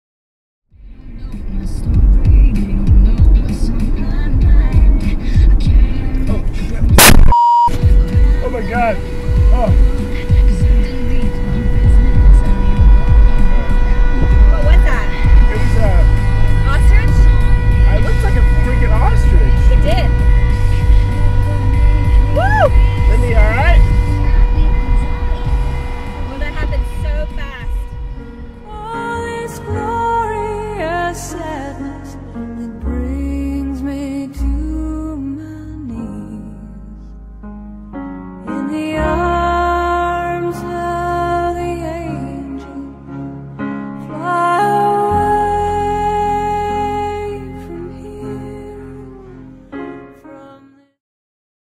Emu Collision on I-85
We were headed back from our family vacation passing through Georgia on I-85 southbound when this happened. Apparently it had escaped from a farm a few weeks prior.

He finally got to fly.